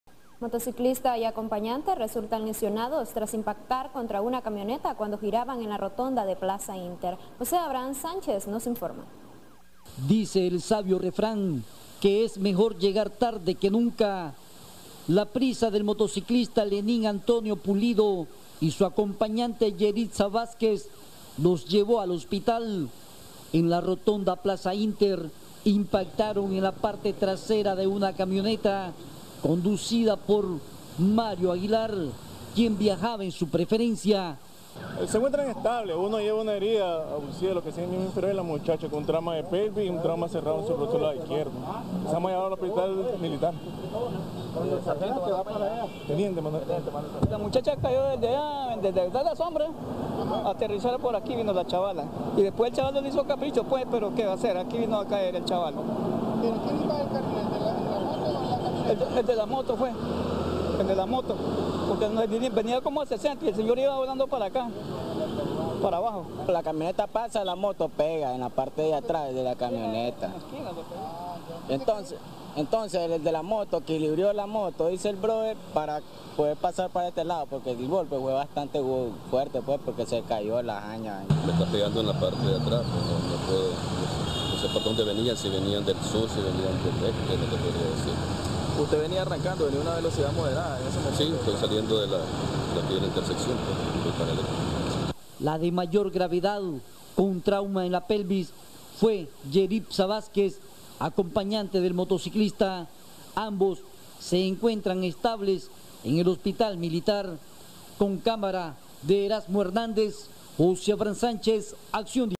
Dos personas resultan lesionadas tras accidente de tránsito
Un motociclista y su acompañante resultaron heridos tras impactar en la parte trasera de una camioneta. Cuerpos de socorro atendieron la emergencia